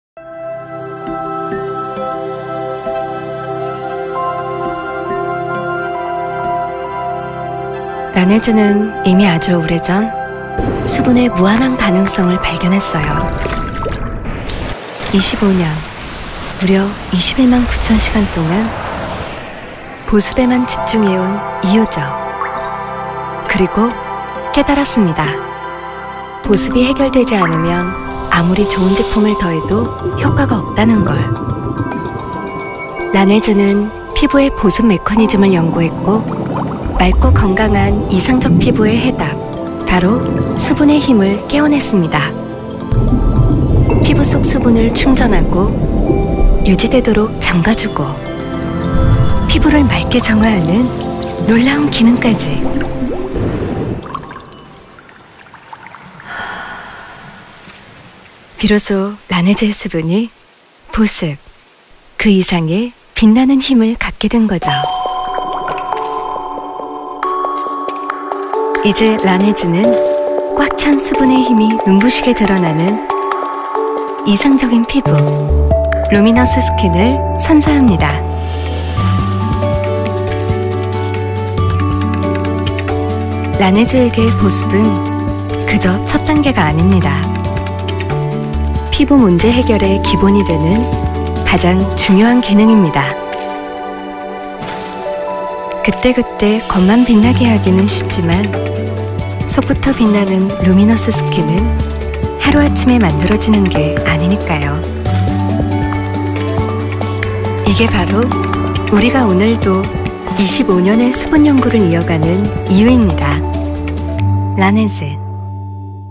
LANEIGE MOISTFUL LUMINOUS SKIN
LANEIGE WATER BANK ESSENCE & CREAM มอยส์เจอร์ไรเซอร์
ชนิดเอสเซ้นส์และครีมที่อุดมด้วยส่วนผสมทรงประสิทธิภาพเข้มข้นจาก
Green Mineral Water ที่ได้จากการผสานพลังแห่งความชุ่มชื้น
จากผัก 6 ชนิด และน้ำแร่จากใต้ทะเลลึก เพื่อเติมเต็มความชุ่มชื้นให้ผิวอย่างล้ำลึก พร้อมล็อคความชุ่มชื้นไว้ในผิว ให้ผิวชุ่มฉ่ำล้ำลึกยาวนานตลอดวัน!!